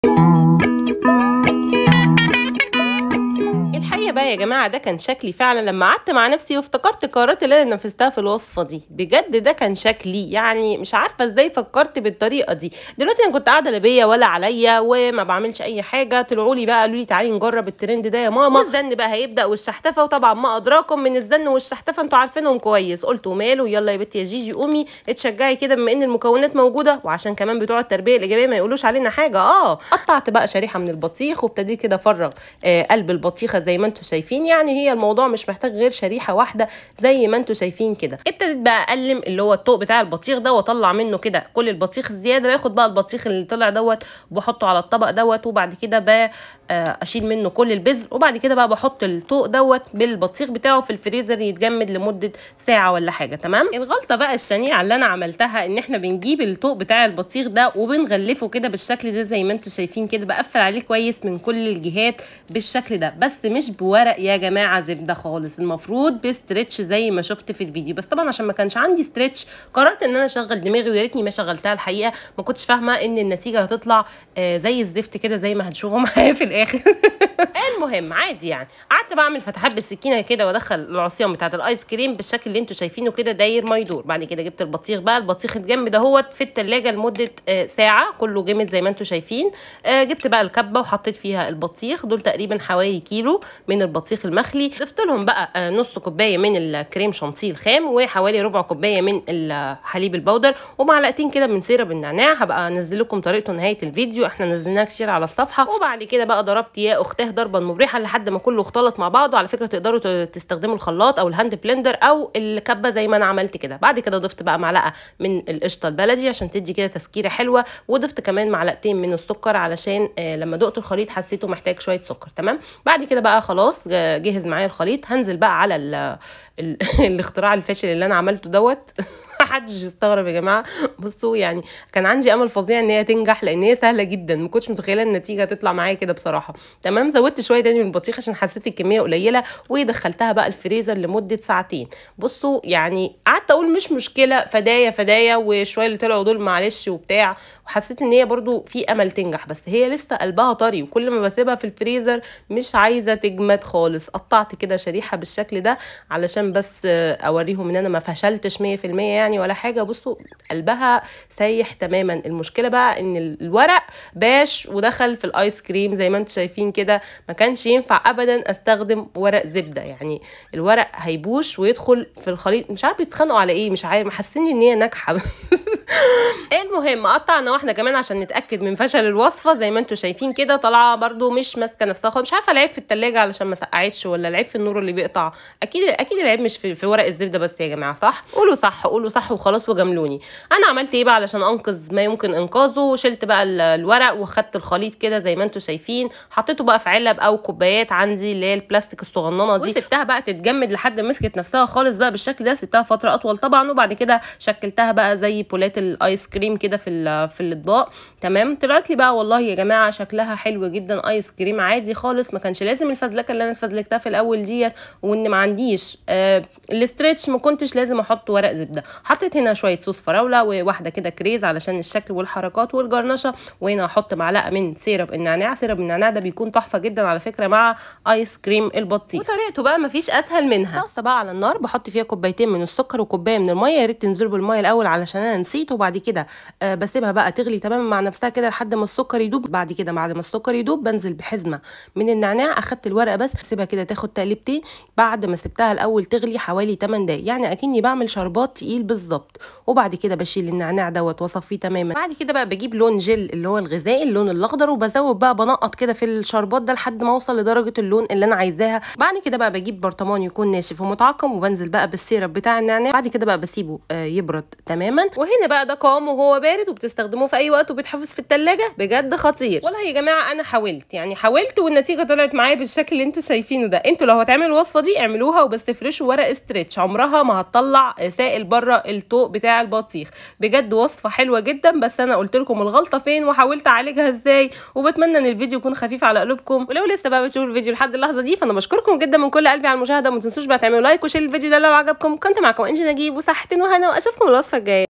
ترند جديد تحت الفظيع (تحت مش فوق لا) انا مش ندمانه ولا حاجة 🤦‍♀️
المقادير :- ☺️🤝
كيلو بطيخ (اي نوع فاكهة ينفع)
نصف كوب شانتية
ربع كوب حليب باودر
٢ م.ك شربات نعناع
٢م.ك سكر
م.ك قشطة

ماعليك سوي الانتساب لهذه القناة لمشاهدة المزيد من الوصفات وللاستفادة من المزايا:-

كواي-kwai :-
‎ينشئ مدون الفيديو هذا مقاطع فيديو رائعة! تعالي وشاهد!